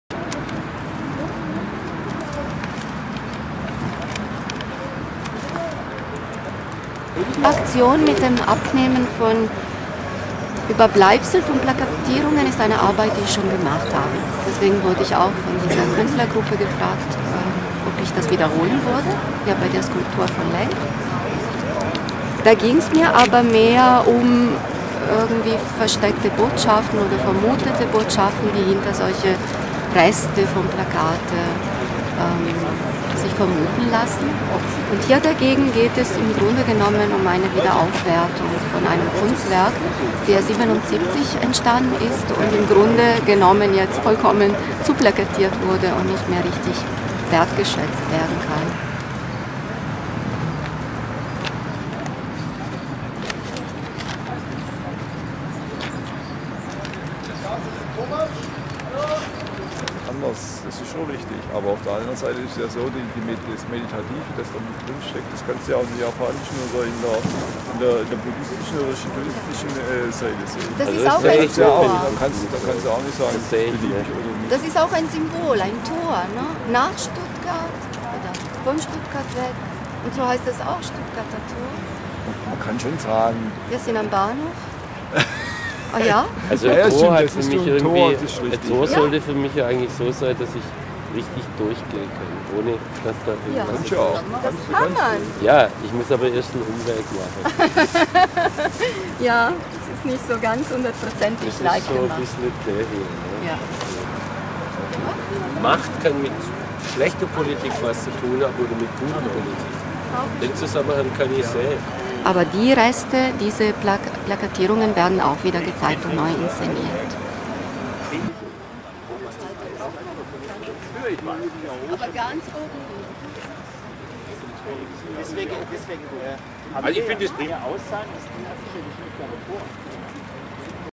Stuttgarter Tor.mp4
Die Künstlerin Maria Grazia Sacchitelli befreit das Stuttgarter Tor von Plakatierungen, um sie zu konservieren und die Skulptur von Lenk wieder in den ursprünglichen Zustand zu versetzen